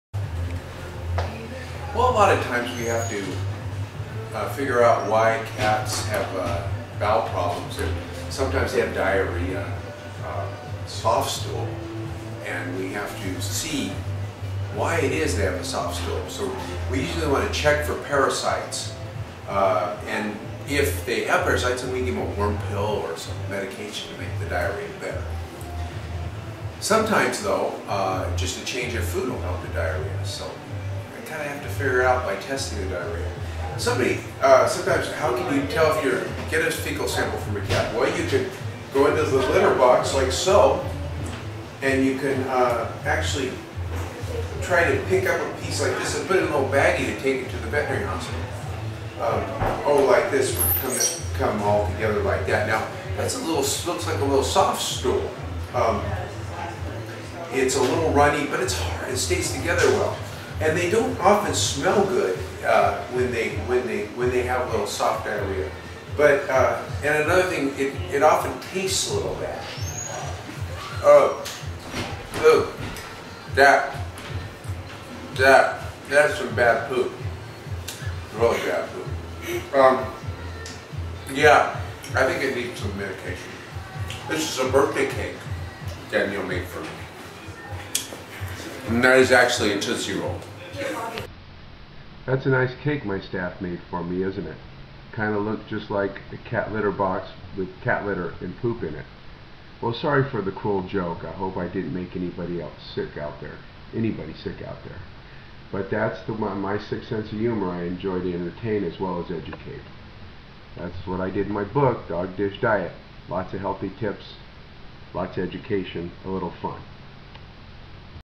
Diarrhea in Cats
Dr Greg talks about stool samples,diarrhea, and treatment in cats